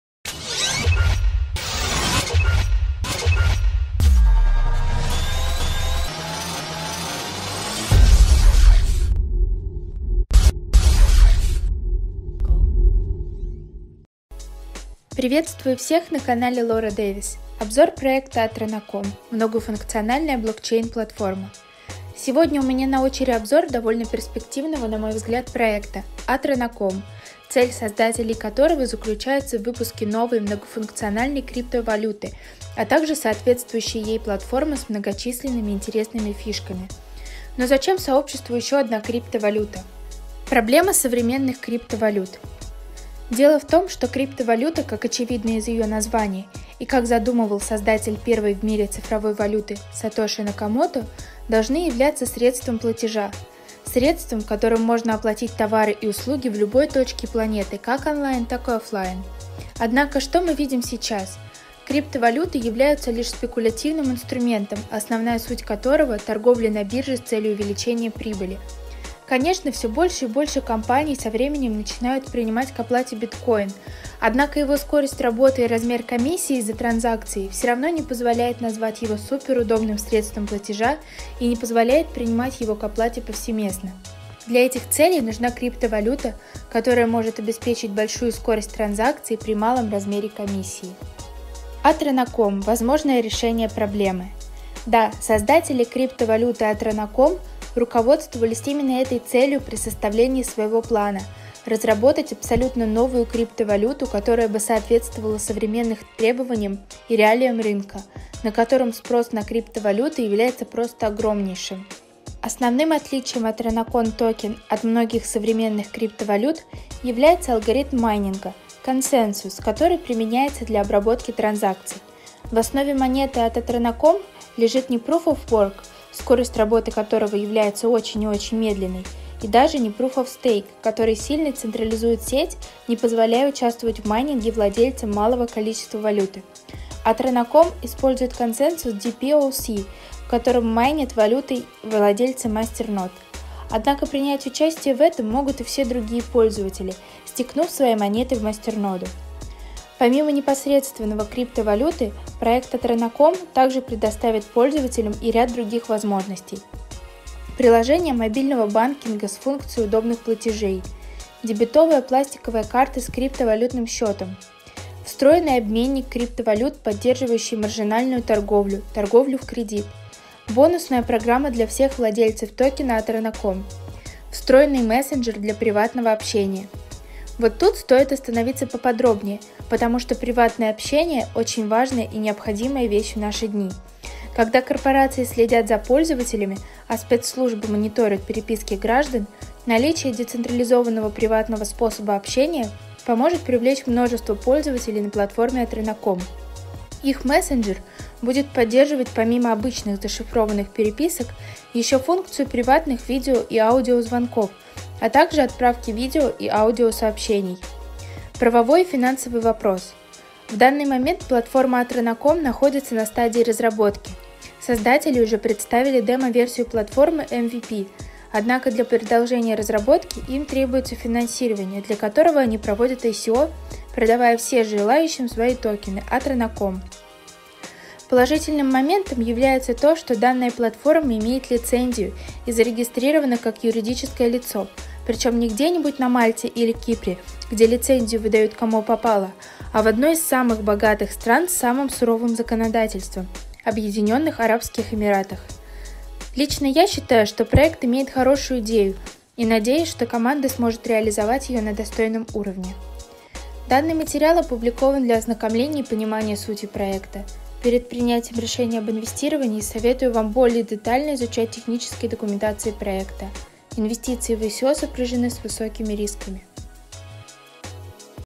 Обзор проекта ATRONOCOM – многофункциональная блокчейн платформа
P.S.: Данный материал опубликован для ознакомления и понимания сути проекта.
Перед принятием решения об инвестировании советую вам более детально изучать техническую документацию проекта.
Инвестиции в ICO сопряжены с высокими рисками.
Связаться со мной (Telegram) - @Lora Davis